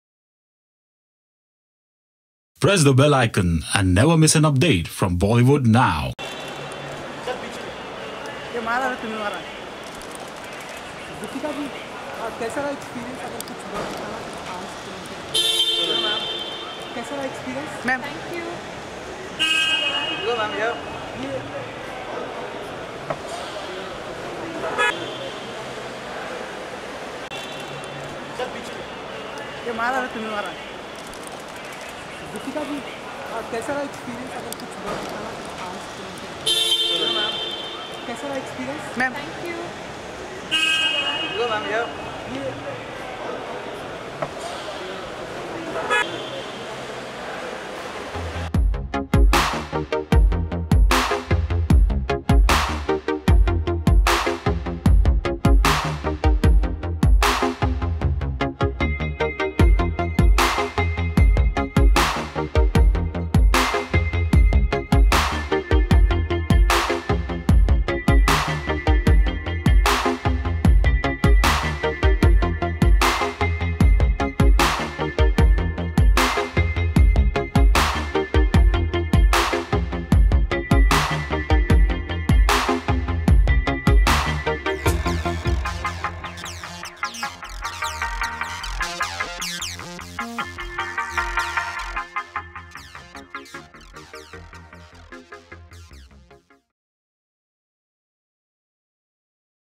DEEPIKA PADUKONE Avoids Reacting On Her Cannes Film Festival 2017 Experience - Returns To Mumbai
After her stunning debut at the Cannes Film Festival 2017, Deepika Padukone is back to her homeland India and avoids reacting on it. Media spotted the diva at Mumbai Airport